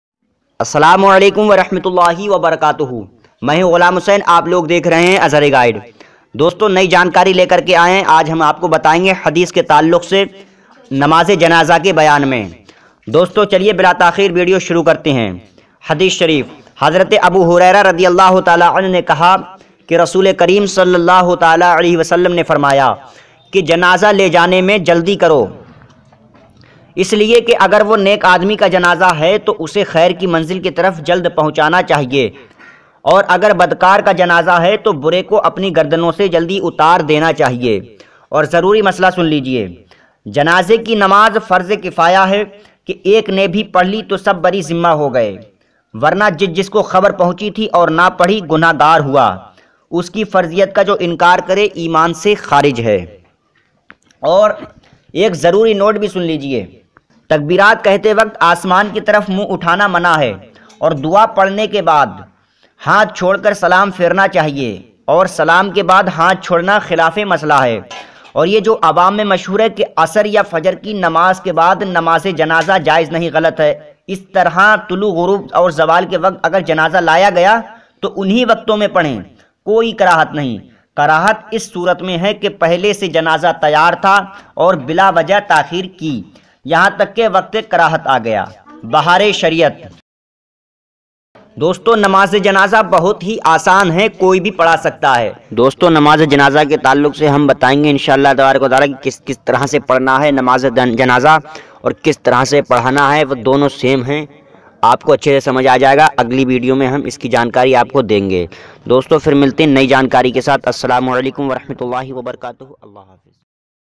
Namaze Janaza ka Bayan | इसे ज़रूर सुने हिदायत के मोती
hadees ke taalluq se janaze ke bayan me kuchh zaruri baten aur zaruri not